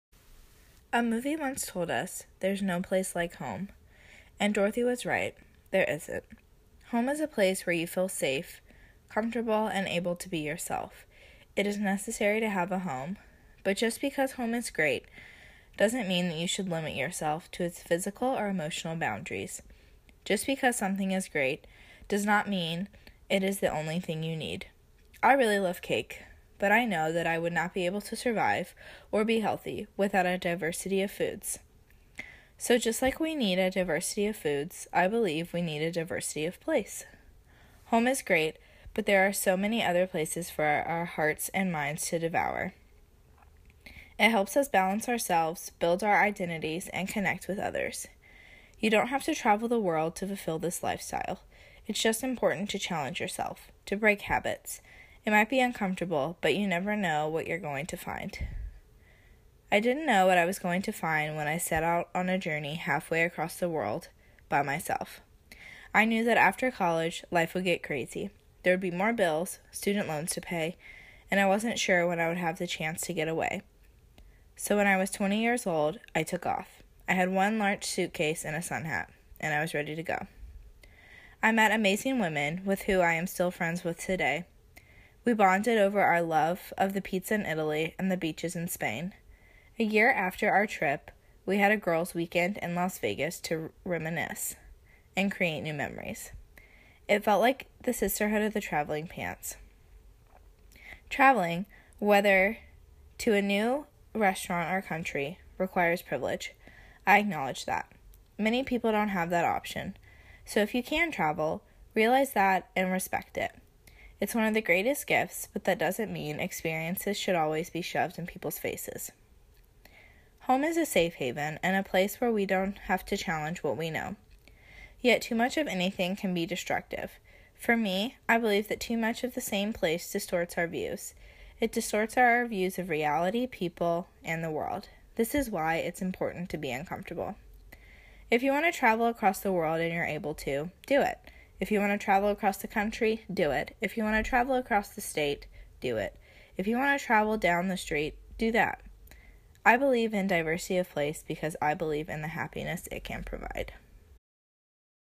Keeley Dority: Love home but cherish travel (2017)
Travel is the challenge that gives us happiness. Created as part of the "This I Believe project" of the Missouri School of Journalism Editorial Writing Class. Clyde Bentley, professor.